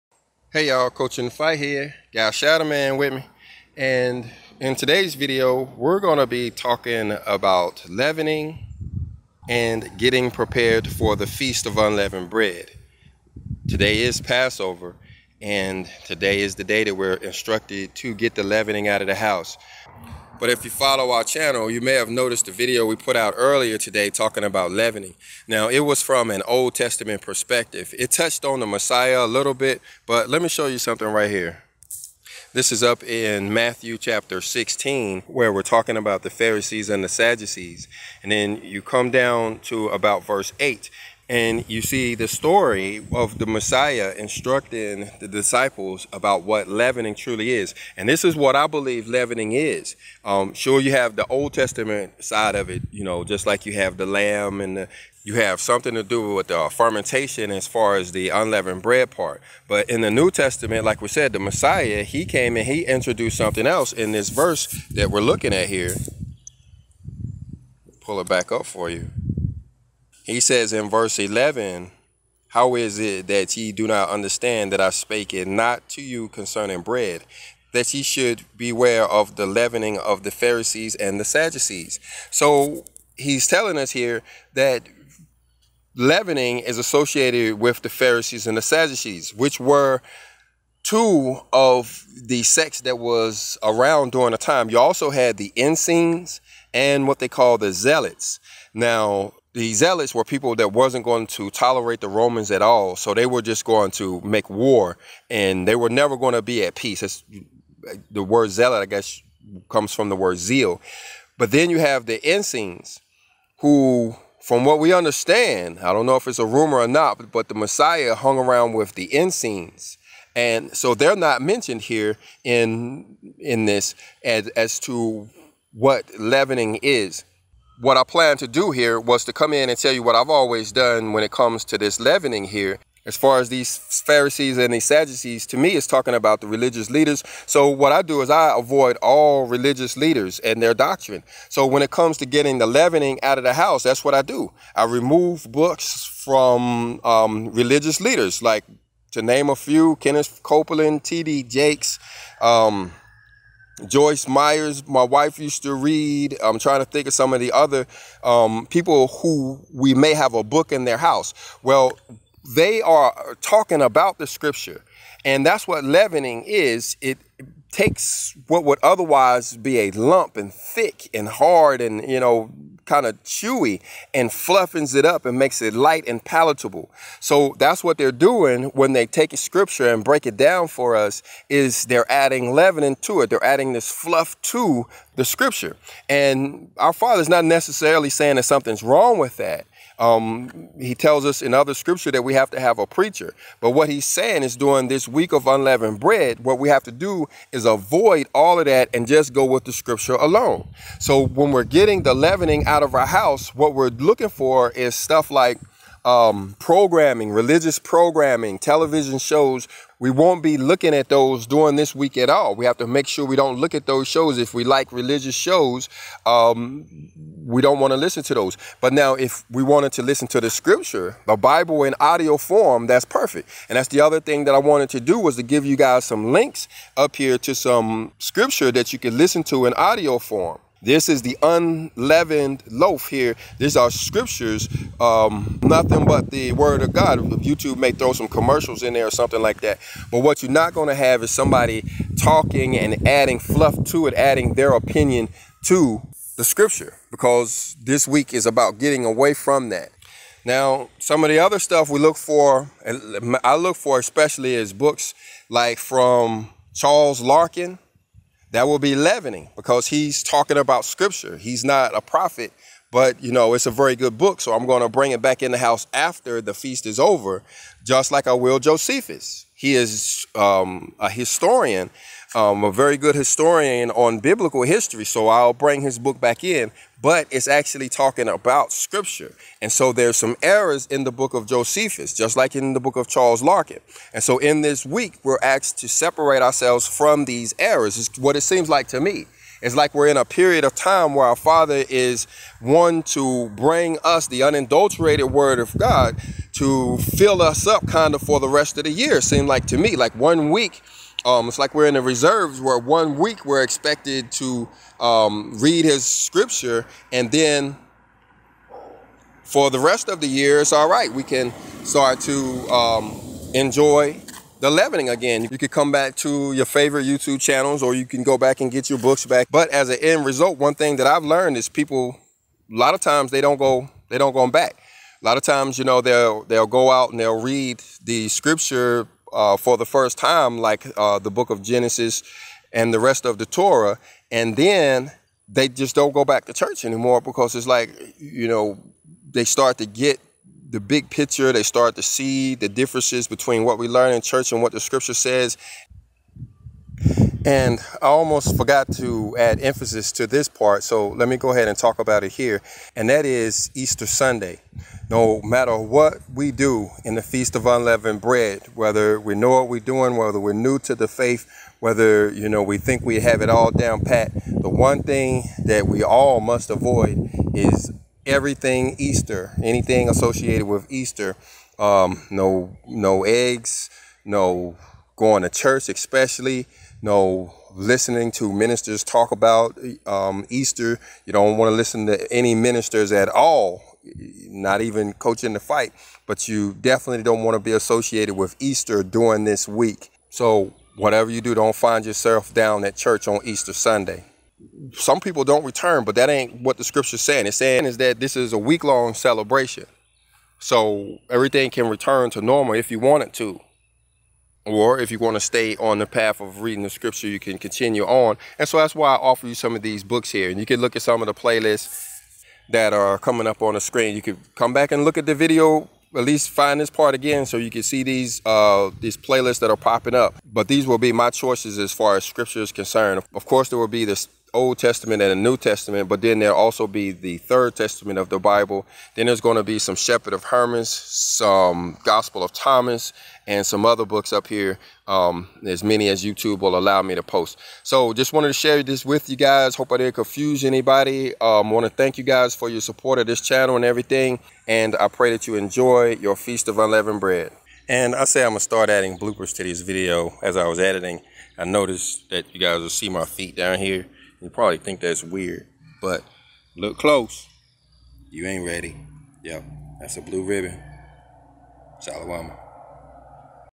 Unleavened Bread Observance Suggestions
this is a follow-up to a video we did earlier on getting the leavening out our houses. in this video we go into detail as to what exactly leavening is and what it is that we have to take out of our houses and avoid.
Pharisee Sadducee Doctrine Yeast Vinegar Sin 666

Help with The Hillbilly Homestead.

Coach inthefight
Repton AL  36475